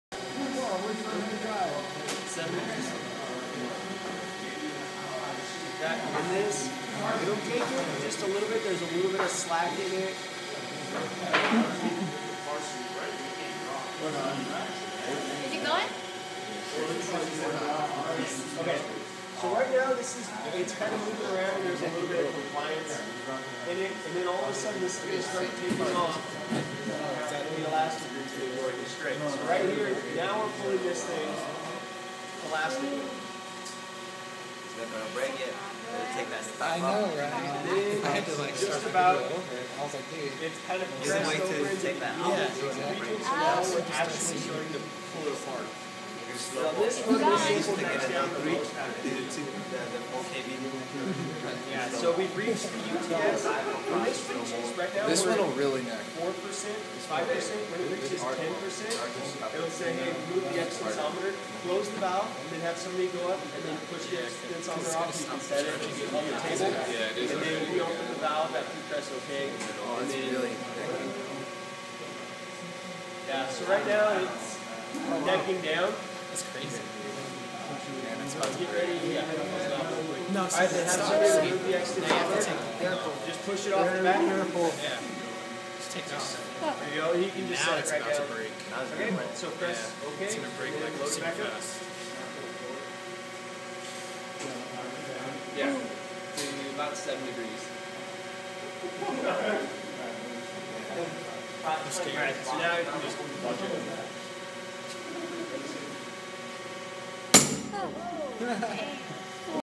Steel Tensile Test
This is a tensile test of Fully Annealed 4140 steel. The steel fails at 1:47. About 17,000 pounds of force was applied to pull this steel sample apart. It stretched almost a half inch. This was for the Engineering Materials (ENGR 45) class at Sacramento State.